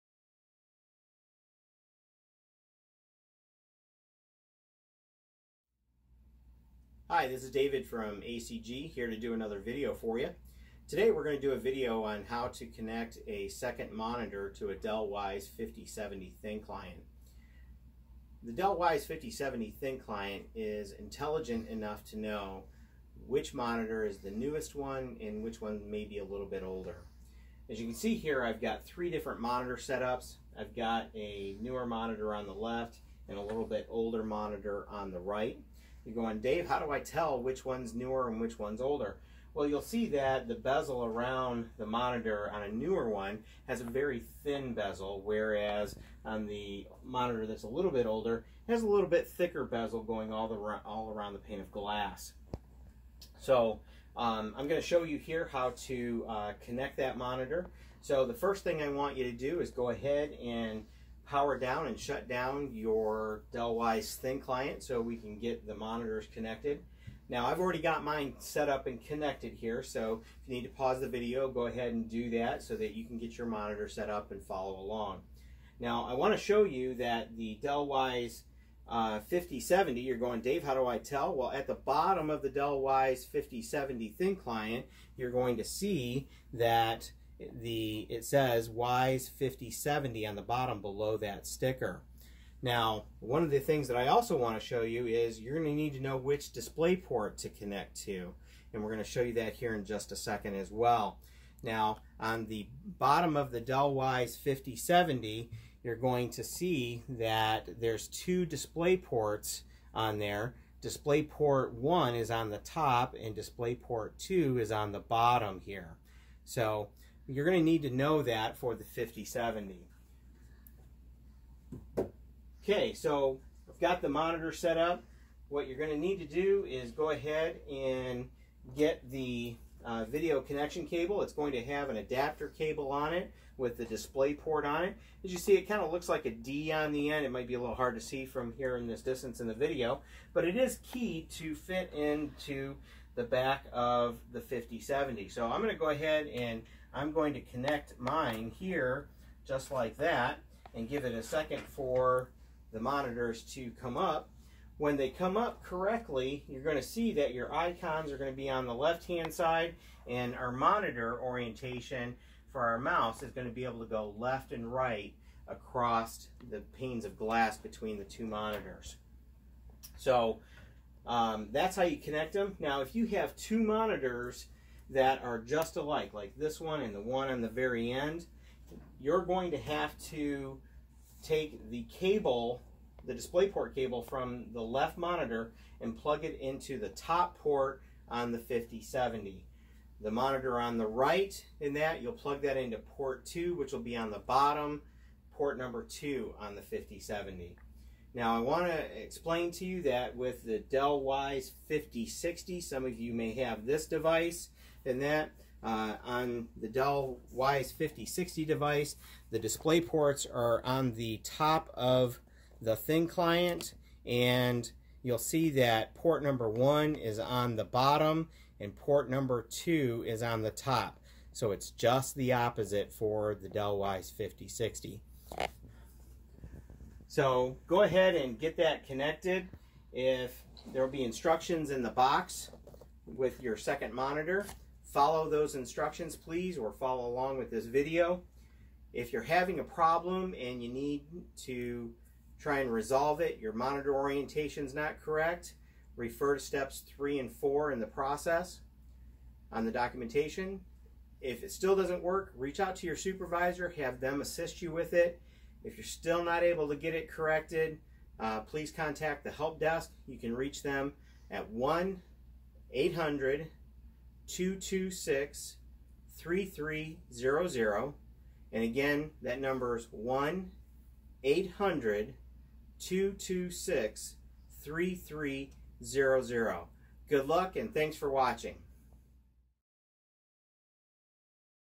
2nd Monitor Setup
2 Monitor Setup on Dell Wyse Thin Client 5060 & 5070